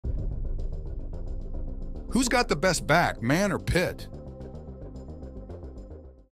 Man vs Pitbull Who Will Win?
"Muscle Showdown: Man vs Pit Bull Terrier | Who Wins the Muscular Appeal?

Get ready for the ultimate muscle showdown! In this video, we compare the impressive physique of a man against the raw power of a Pit Bull Terrier. Watch as we delve into the muscular appeal of both contenders, highlighting their strength, agility, and sheer presence